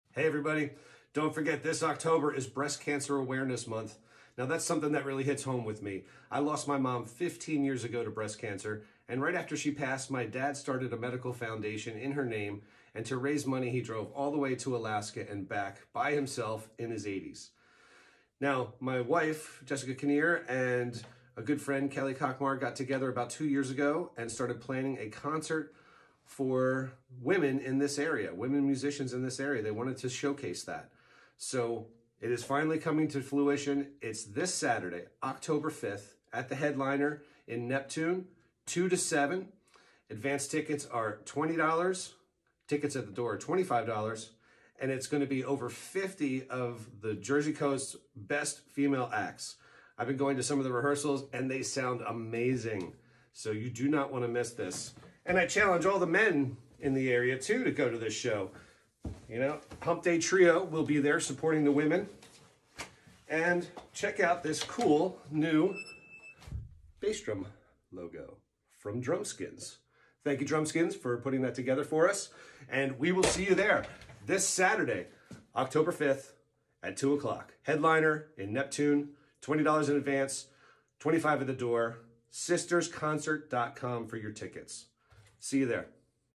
Sisters for a Cure at the Headliner Neptune NJ 10/05/24. sistersconcer.com
Over 50 of the Jersey Shore’s best female artists will be performing at the Headliner in Neptune NJ on Saturday October 5th at 2:00 to 7:00. Advance tickets $20 or $25 at the door. All ticket sales will go to Susan G. Komen Foundation.